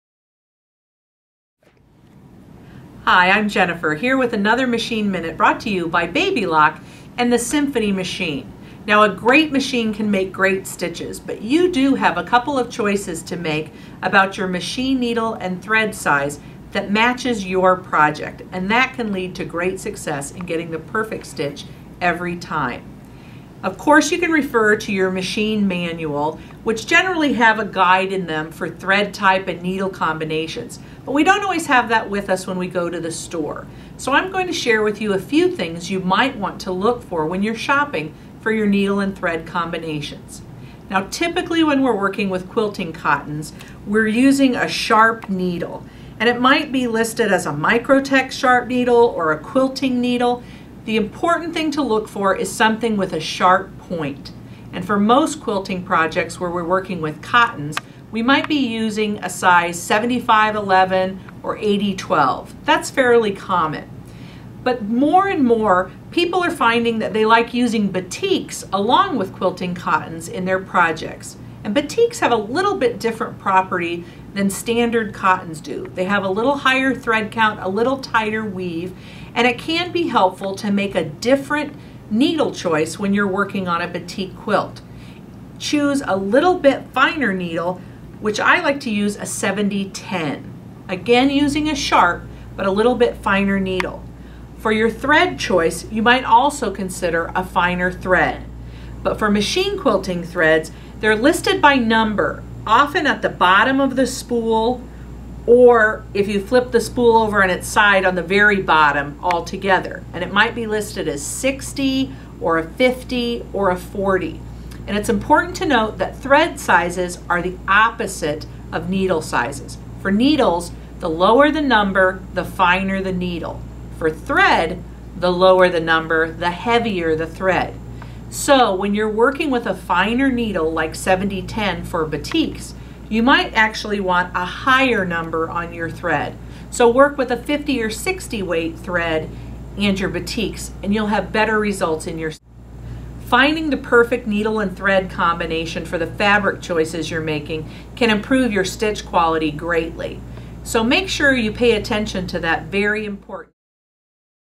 Machine Minute - Needle and Thread Combos: Batiks
Choosing the correct needle and thread combination results in great stitches. Learn how to select the right needle and thread for sewing batiks.